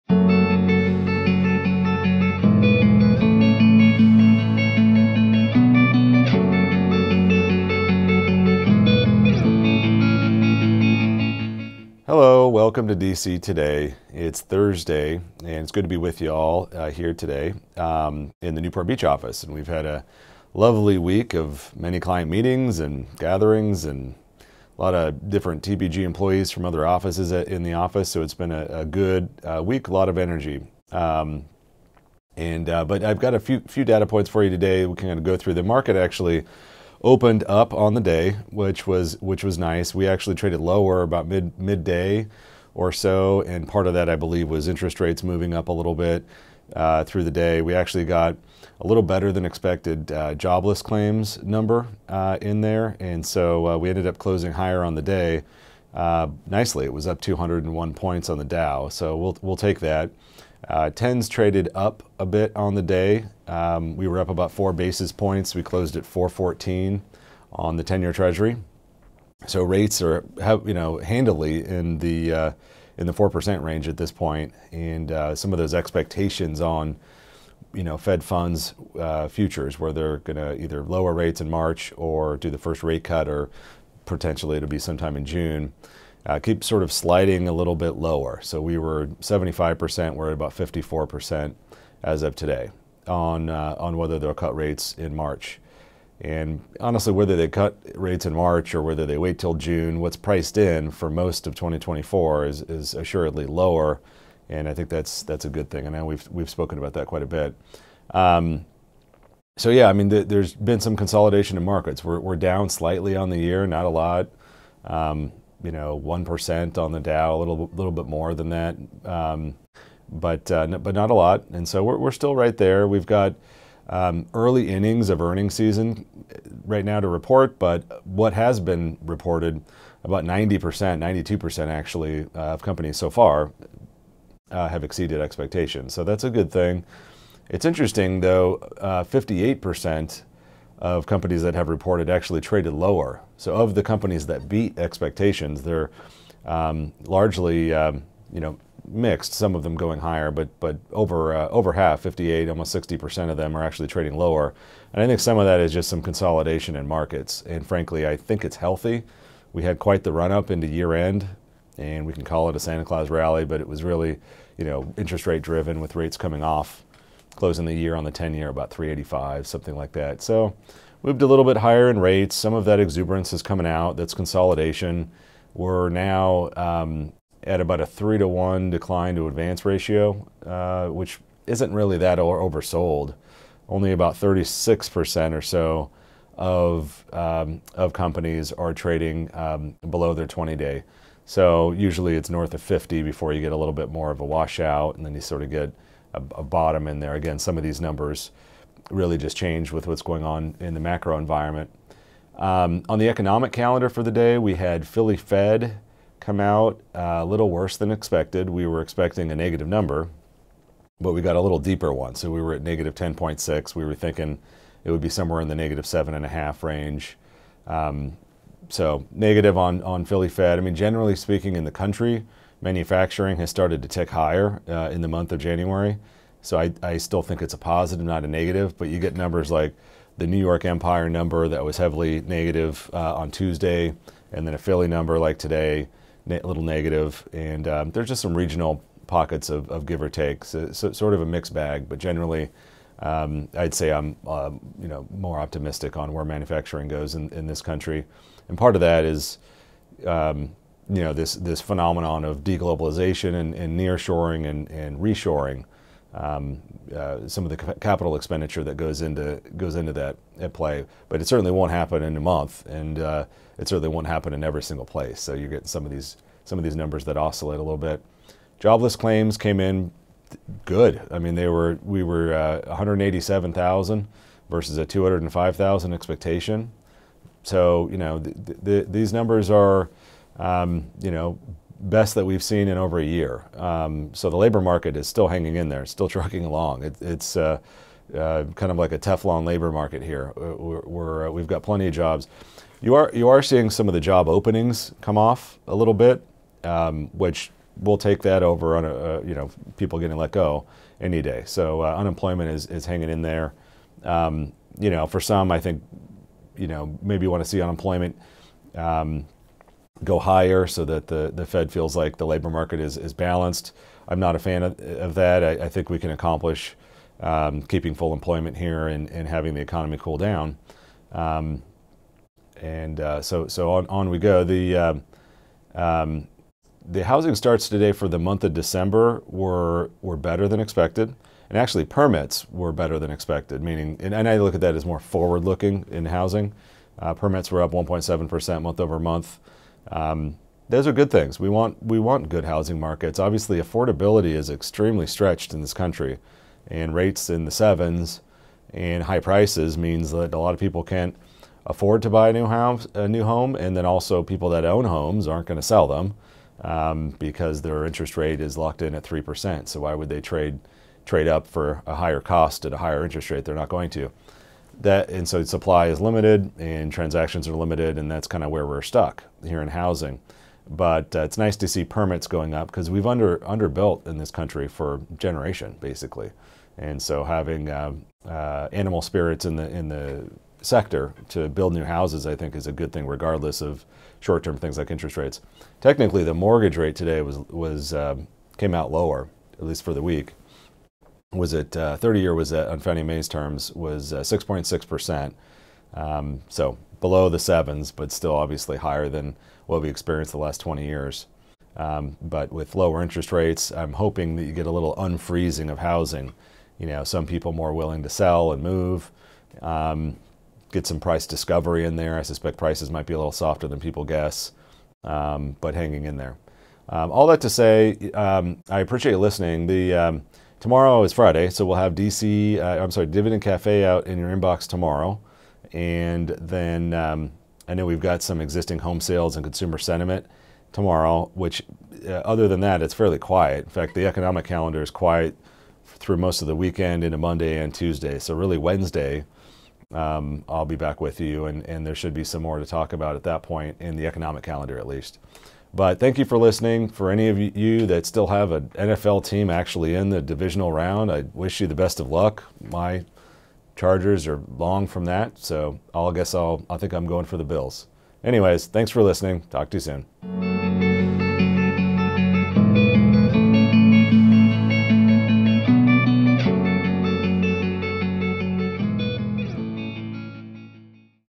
The DC Today - Thursday, January 18, 2024
A positive morning of trading in markets gave way to losses mid-day, only to gain it all back and then some as we headed towards the close.